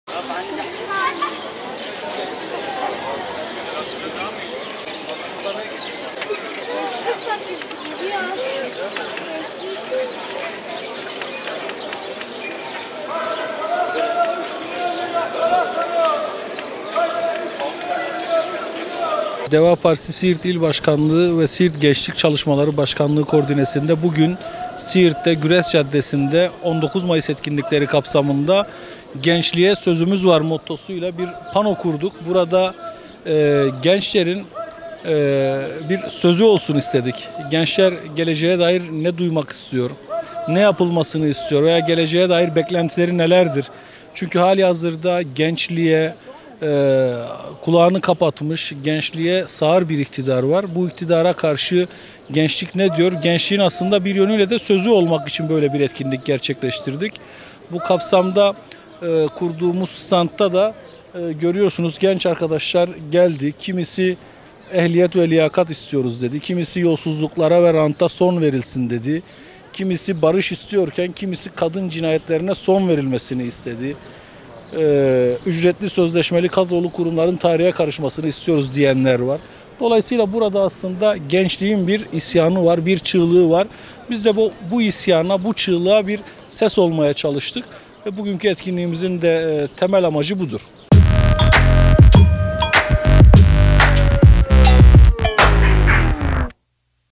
"GENÇLİĞİN BİR İSYANI VE ÇIĞLIĞI VAR"
Demokrasi ve Atılım Partisi Siirt İl Başkanlığı ve DEVA PArtisi Siirt Gençlik Çalışmaları Başkanlığı koordinesinde Siirt’in Güres Caddesinde bir etkinlik gerçekleştirildi.

Etkinlikte Güres Caddesine “Gençliğe Sözümüz Var” sloganıyla yerleştirilen panoya gençler talep ve isteklerini yazdı.

Etkinliğe katılan DEVA Partisi Siirt İl Başkanı Umut Dayanan Artı Siirt Mikrofonlarına yaptığı açıklamada, “Gençliğin bir isyanı var bir çığlığı var bizde bu isyana ve çığlığa bir ses olmak istedik” dedi.

“Gençliğe kulağını kapatmış gençliğe sağır bir iktidar var.” diyen Dayanan konuşmasının devamında ise şunları söyledi.

“DEVA Partisi Siirt İl Başkanlığı Siirt Gençlik Çalışmaları Başkanlığı koordinesinde Bugün Güres Caddesinde 19 Mayıs etkinlik kapsamında “Gençliğe Sözümüz Var” notuyla bir pano kurduk. Burada gençlerin bir sözü olsun istedik, gençler geleceğe dair ne duymak istiyor, ne yapılmasını istiyor veyahut geleceğe dair beklentileri nelerdir çünkü hâlihazırda gençliğe kulağını kapatmış gençliğe sağır bir iktidar var.  Gençliğin bir nevi sözü olmak için böyle bir etkinlik gerçekleştirdik. Bu kapsamda kurduğumuz stantta da genç arkadaşlarımız taleplerini dile getirdi. Burada aslında gençliğin bir isyanı var bir çığlığı var bizde bu isyana ve çığlığa bir ses olmak istedik ve bugün ki etkinliğimizin temek amacı da budur.”